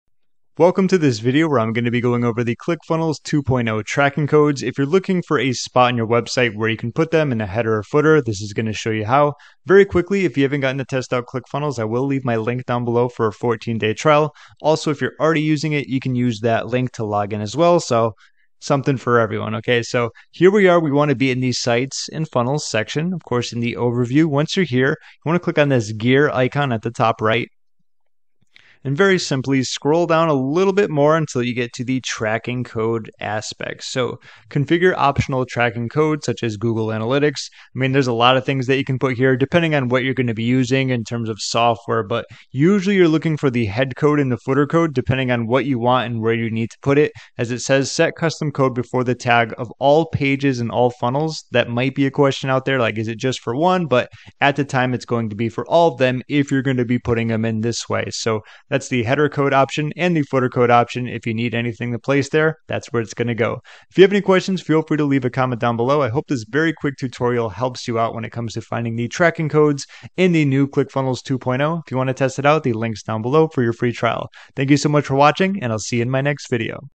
ClickFunnels 2.0 Tracking Codes (How To Add Tracking Code To Website)
ClickFunnels 2.0 Tracking Codes Video

Would you like to see how to add tracking codes to a website? Then this ClickFunnels 2.0 tracking codes tutorial is definitely for you. Check out the video to see what you need to do.